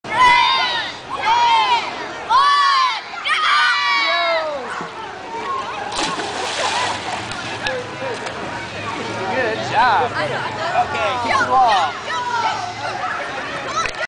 Claire Dives
Claire off the diving board first time age 4